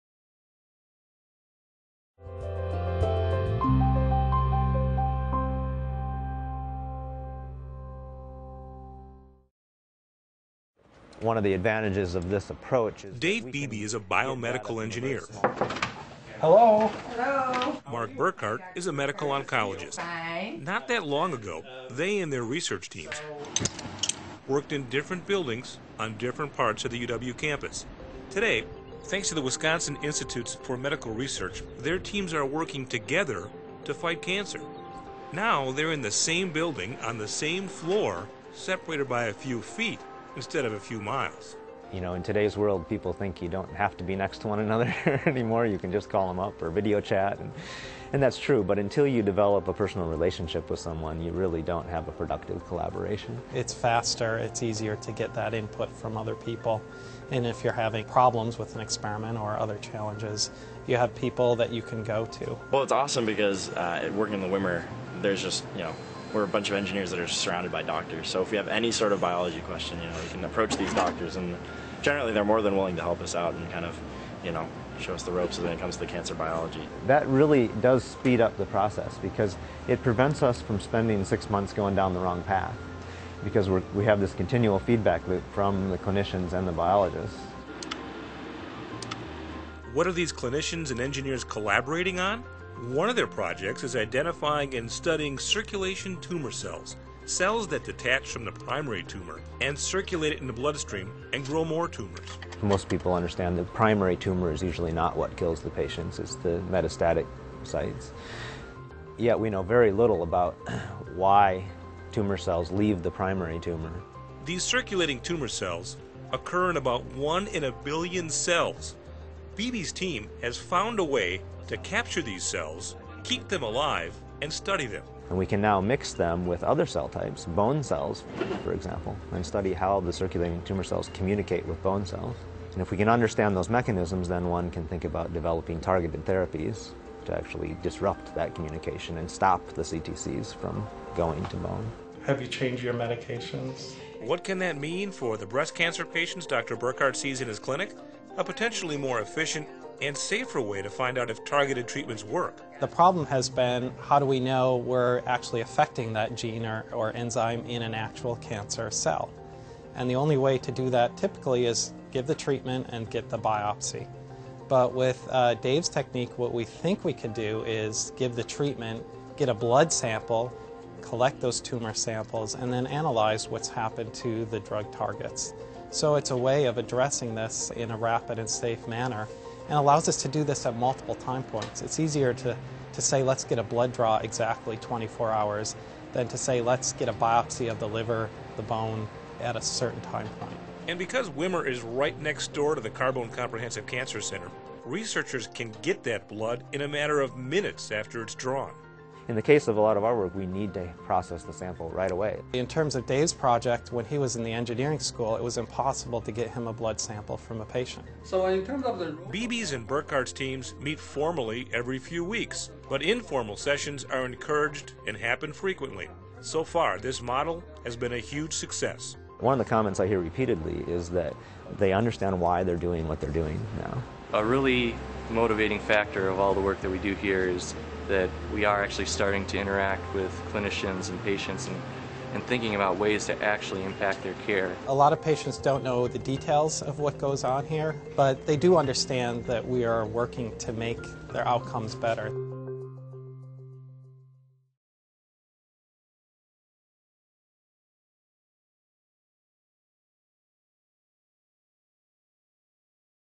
Advancing Cancer Research at WIMR (Prof. David Beebe)
UW Health - University of Wisconsin Carbone Cancer Center - Advancing Cancer Research at the Wisconsin Institutes for Medical Research.  Cancer research is just one example of how the University of Wisconsin School of Medicine and Public Health is bridging the gap between basic science and patient care. Watch the video to learn how collaboration between Dr. Mark Burkhard - a medical oncologist - and Prof. David Beebe -- of the Department of Biomedical Engineering - is improving the care of cancer patients.